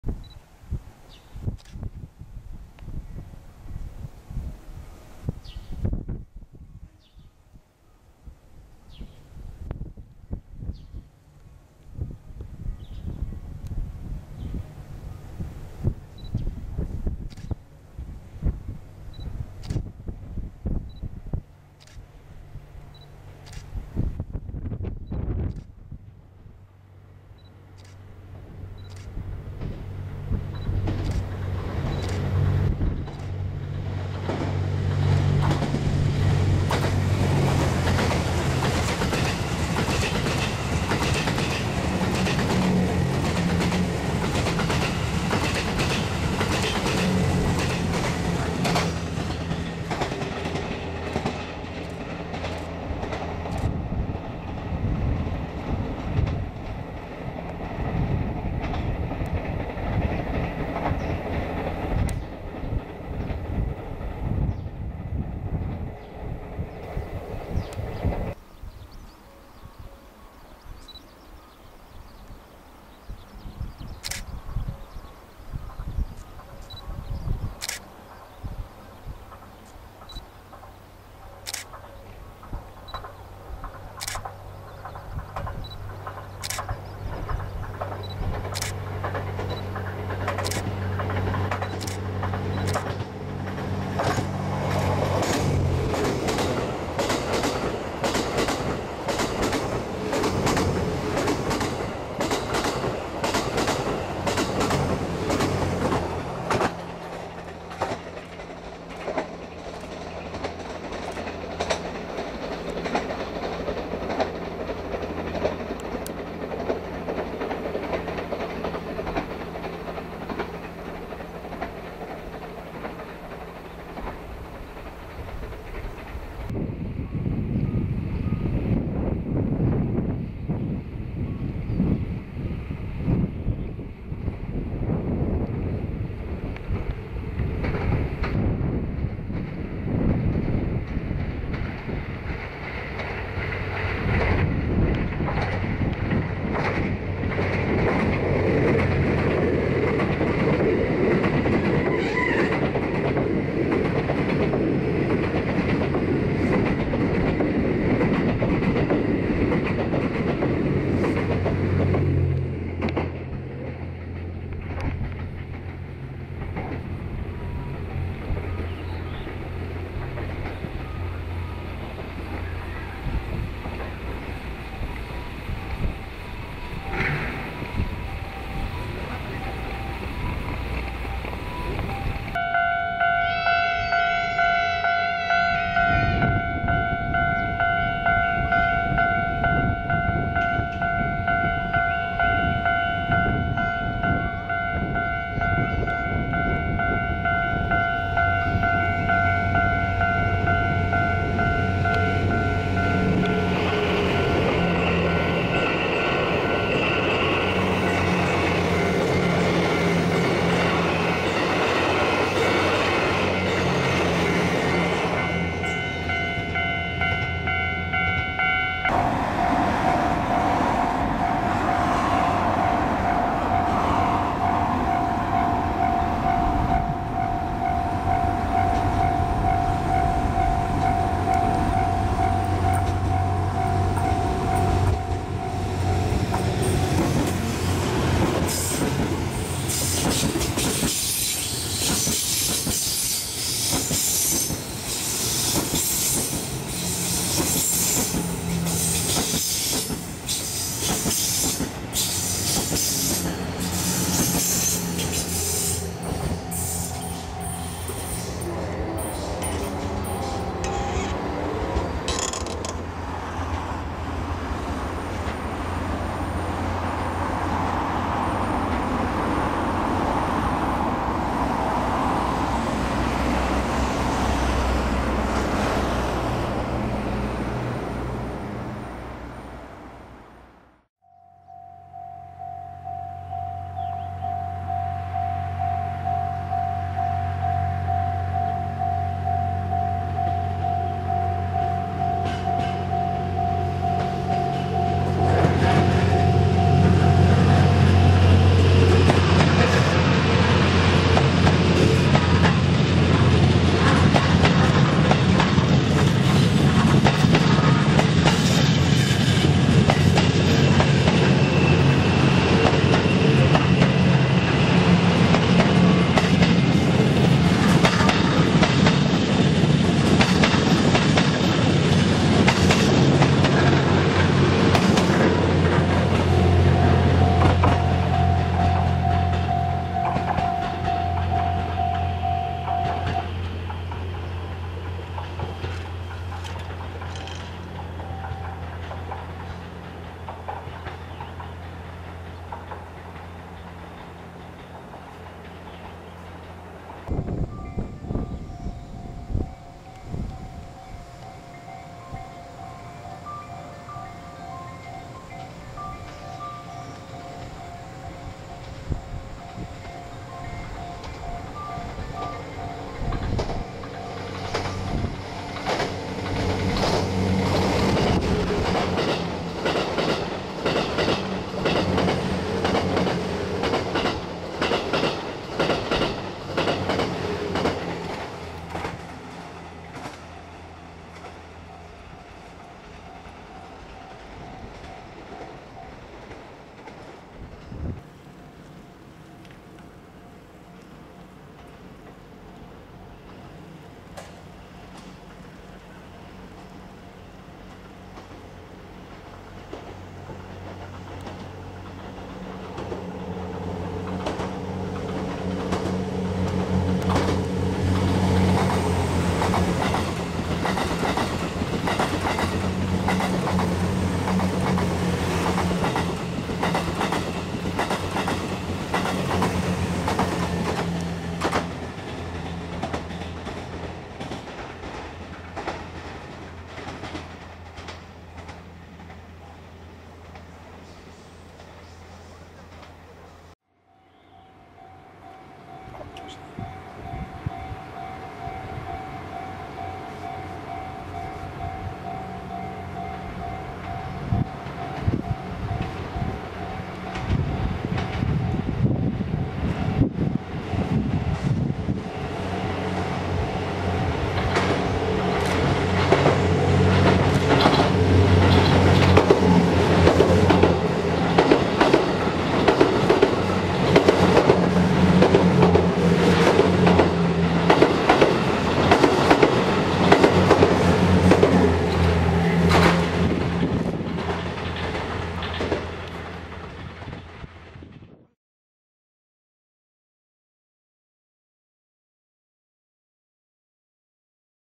【通過編】87系気動車TWILIGHT瑞風の日中の山陰本線10両試運転走行シーンまとめ(かいたくRC撮影)
2017年6月17日のTWILIGHT EXPRESS瑞風デビューを記念して、日中の山陰本線を走る87系気動車トワイライト瑞風の試運転列車走行シーンの通過を1つの動画にまとめました。
動画のすべては過去に私が撮影し、アップロードしたものです。
以下にリンクを貼ります。
山陰本線浜村駅を通過する、TWILIGHT EXPRESS瑞風10両フル編成回送列車（2017/3/19）
【棒線駅に10両編成】山陰本線中山口駅を通過する、TWILIGHT EXPRESS瑞風フル編成回送列車（2017/3/19）
湯町原田踏切と宮廻踏切にて、TWILIGHT瑞風を後ろから撮影（おまけあり）（2017/5/18）
【ノーカット】乃木駅でやくも26号を待たせて通過するTWILIGHT瑞風試運転(2017/6/1)